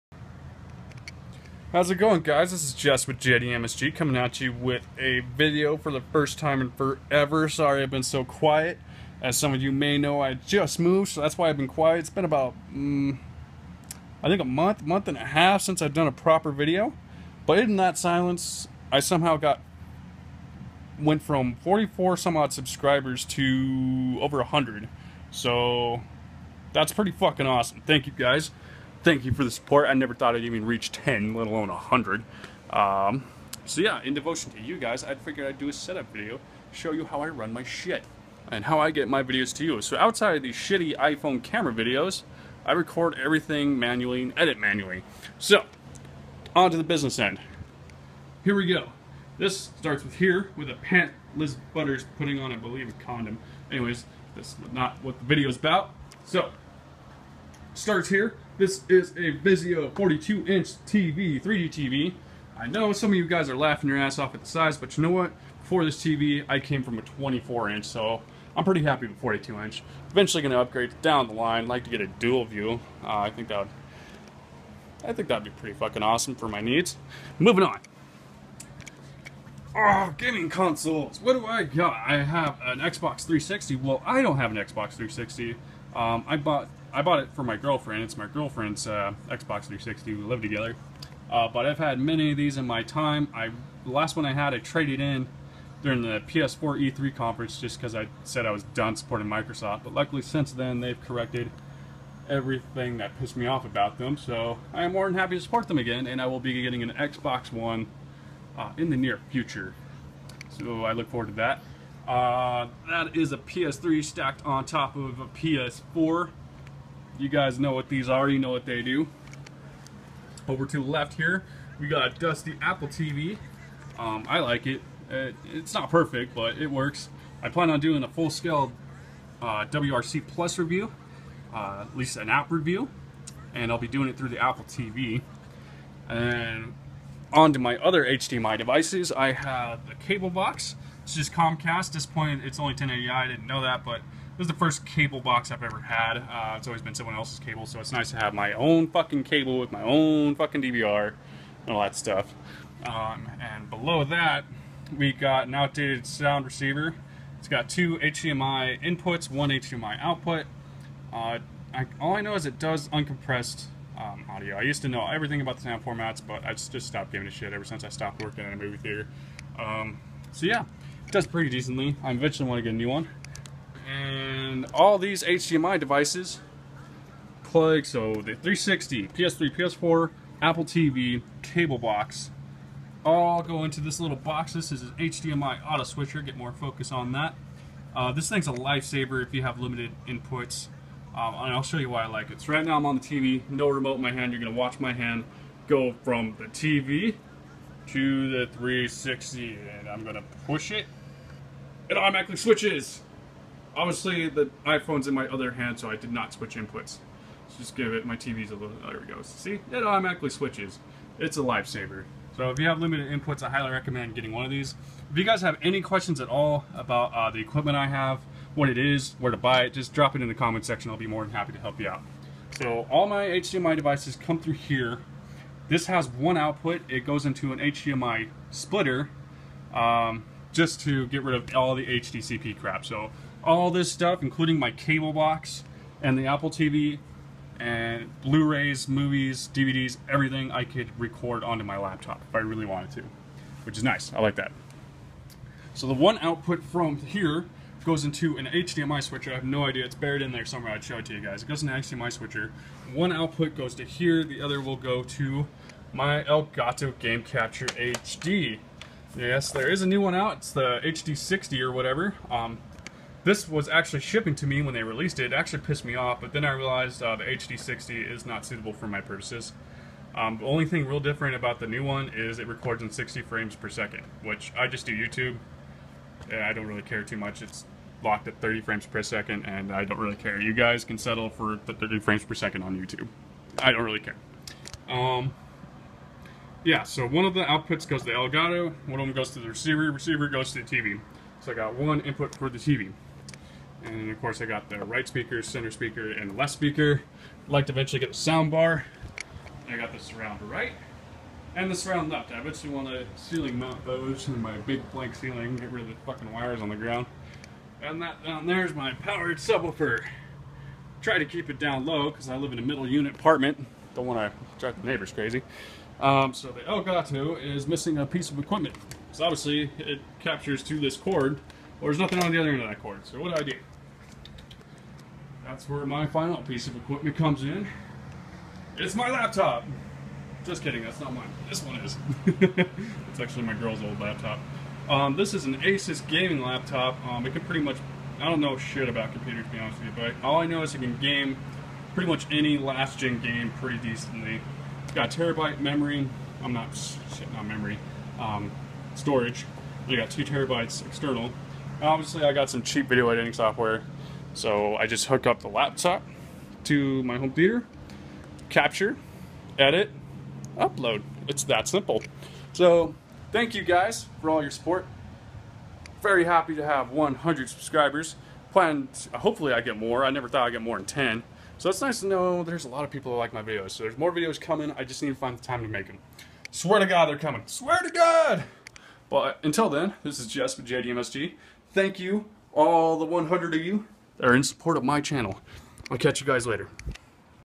My Set Up / 100 Subscribers!
In celebration of getting over 100 subscribers I figured I would show you guys my low end setup and what exactly brings my videos to you guys. Thanks for the subs everyone, they are delicious.